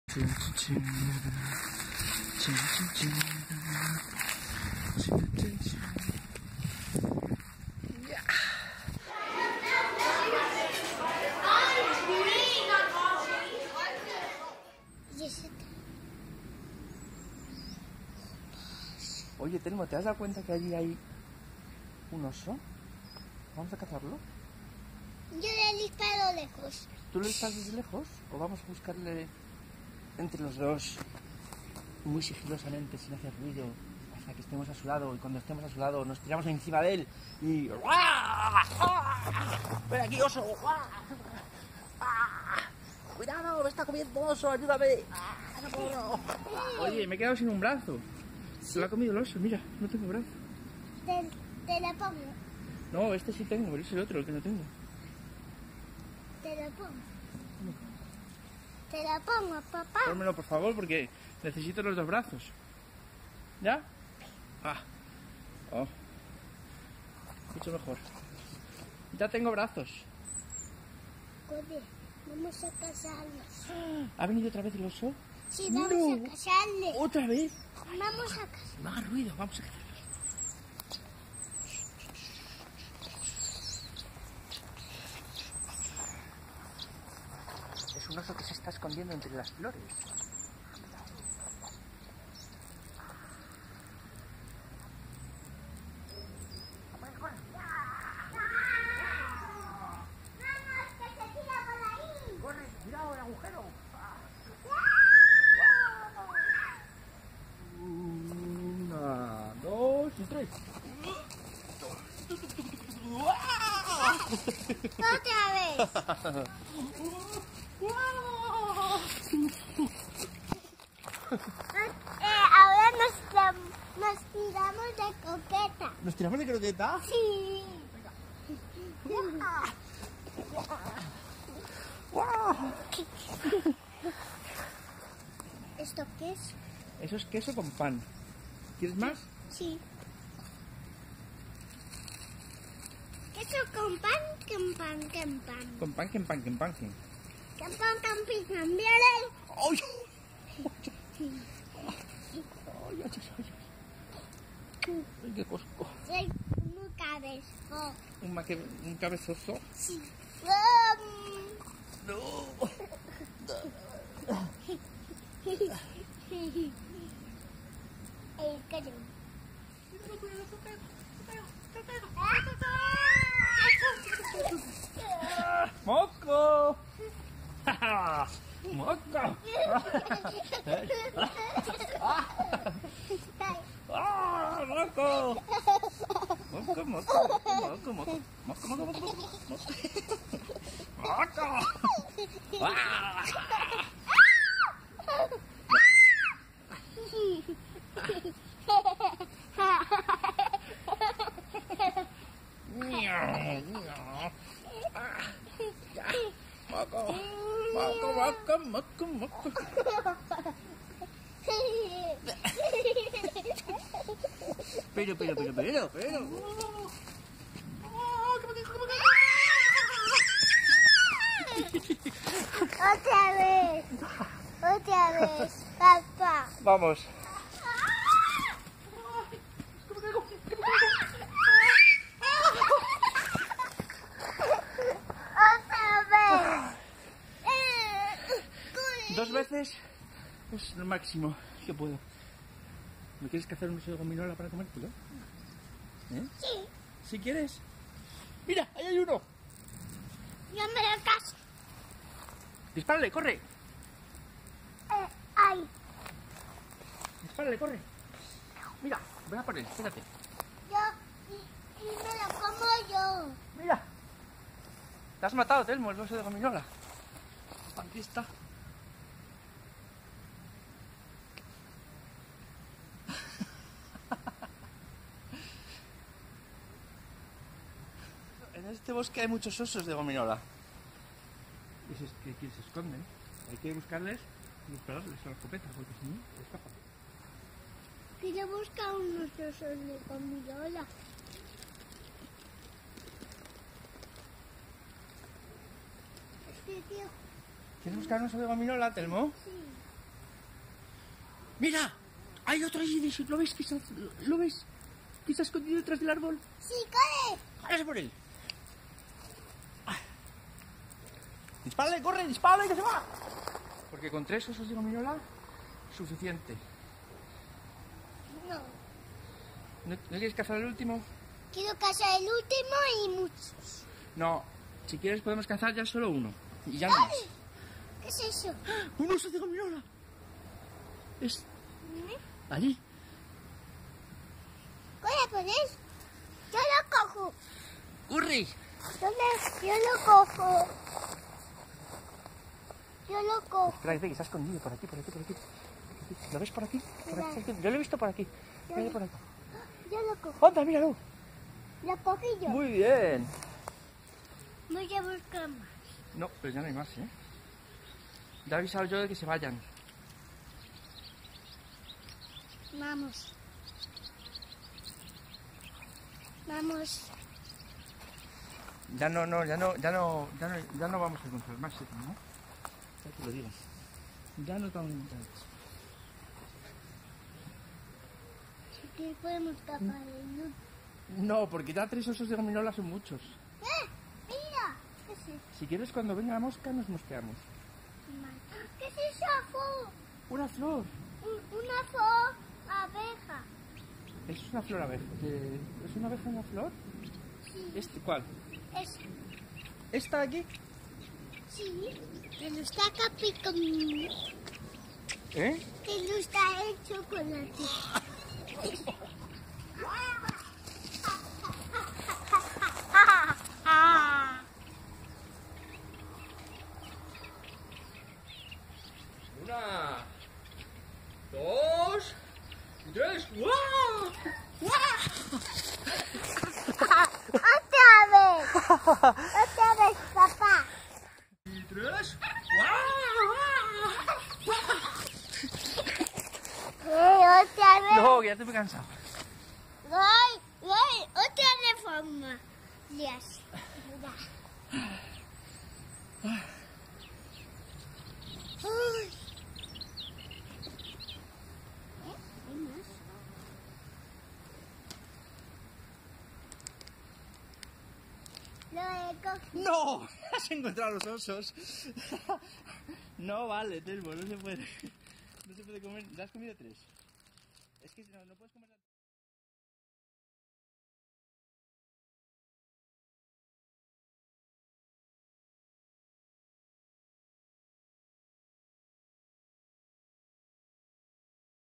Cazando osos de gominola
Telmo descubre la realidad sobre estos huidizos animales